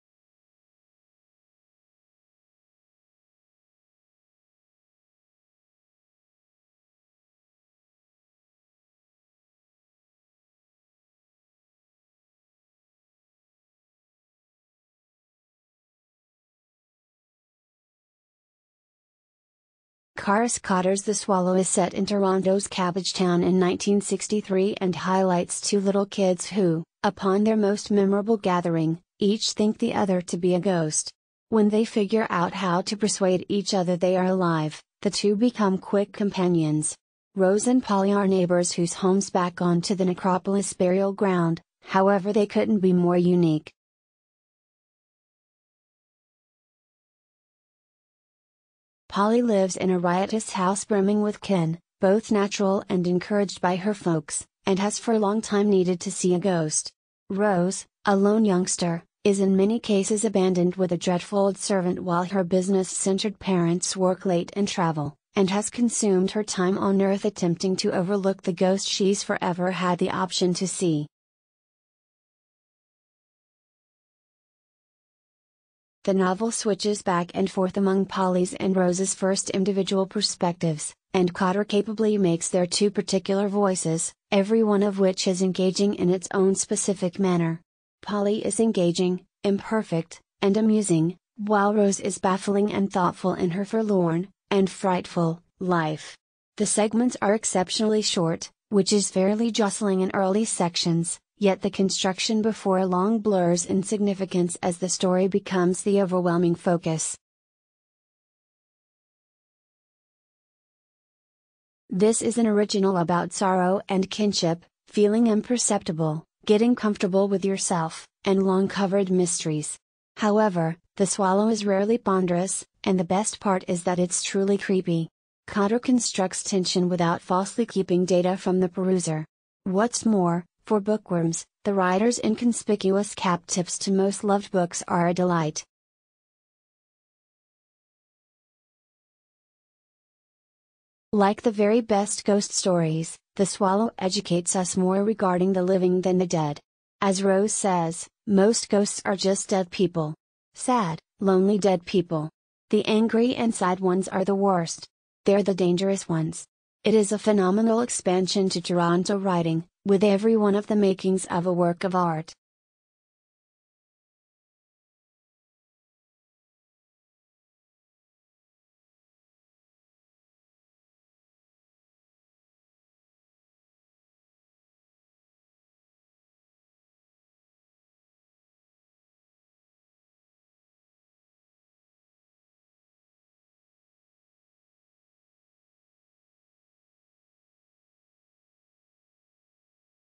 The Swallow (a ghost story)
Charis Cotter's The Swallow is set in Toronto's Cabbagetown in 1963 and highlights two little kids who, upon their most memorable gathering, each think the other to be a Ghost. When they figure out how to persuade each other they are alive, the two become quick companions. Rose and Polly are neighbours whose homes back on to the Necropolis burial ground, however they couldn't be more unique.